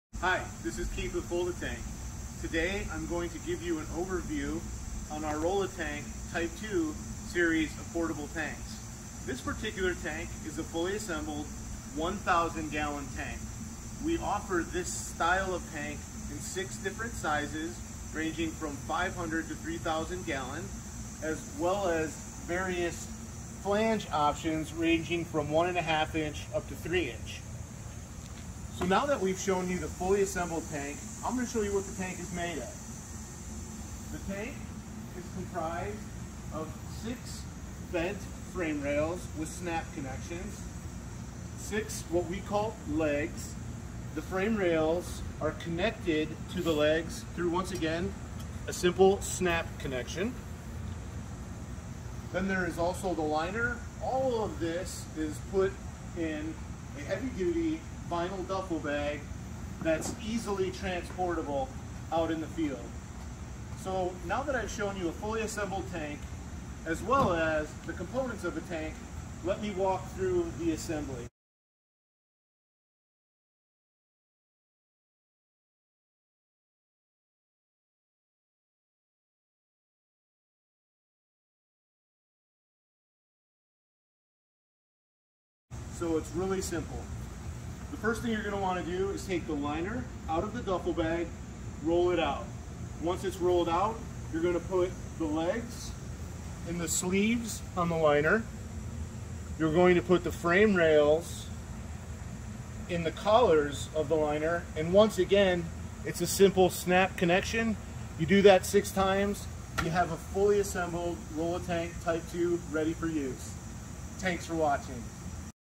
Rol-La-Tank Type II Water Containment Tank
The Rol-La-Tank is a heavy duty water storage tank that packs in a big duffle bag.  It is easy to carry to the point of use and can be assembled quickly by one or two people. The long lasting vinyl or HPR liner comes in many colors fitting standard tank sizes ranging from 500 to 3000 gallons.  Watch Dave and Jeff assemble a tank in high speed.